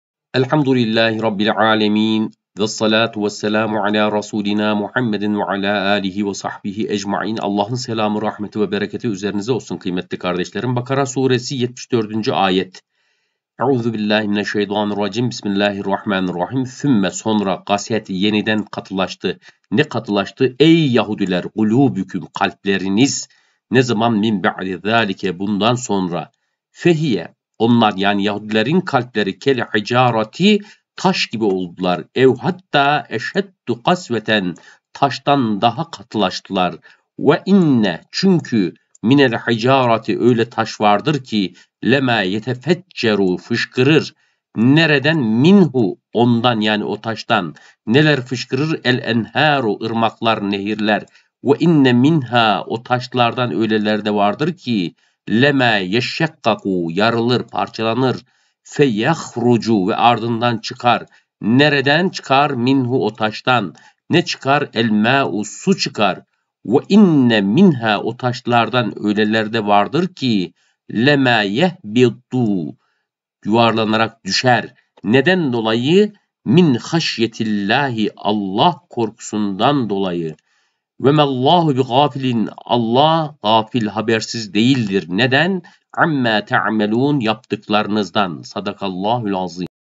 BAKARA SURESİ MEALİ (74. AYET)
KUR'ÂN-I KERÎM KELİME MEALİ
Bakara suresi 74. ayet meali.
ALLAH'ın izniyle yapmış olduğumuz bu çalışmamızda Elmalılı Hamdi Yazır (r.a.), Ömer Nasuhi Bilmen (r.a.), Diyanet ve Ahmet Varol'un meal ve tefsirlerinden istifade edilmektedir.
ALLAH hepsinden razı olsun. Gayret bizden tevfîk ALLAH'dandır